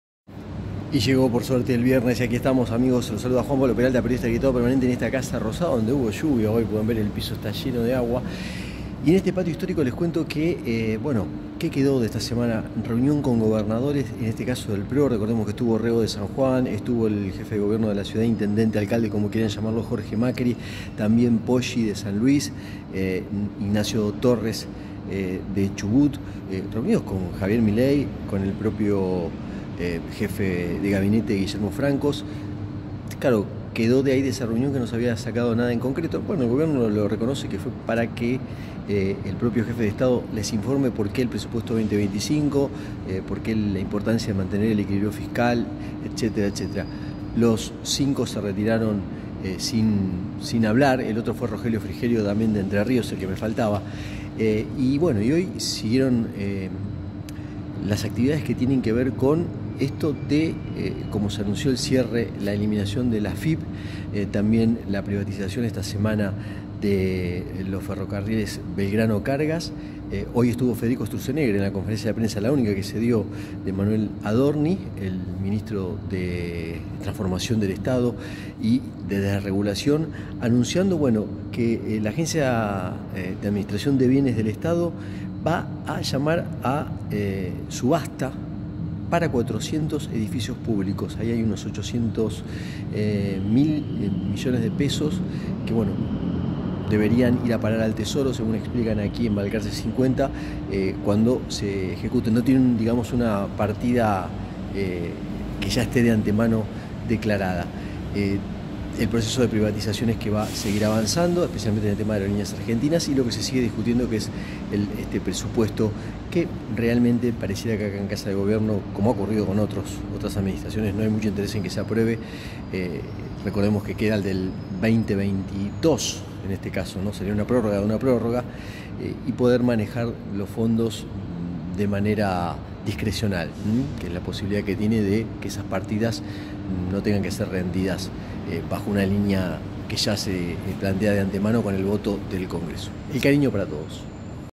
casarosada: SEMANA DE milei CON GOBERNADORES pro, SUBASTA BIENES DEL estado Y presupuesto2025
Todo lo que aconteció esta semana en casarosada está en este informe especial, luego del anuncio de eliminación de la afip, privatización del ferrocarril belgranocargas, subasta de bienes del estado y almuerzo de javiermilei con gobernadores del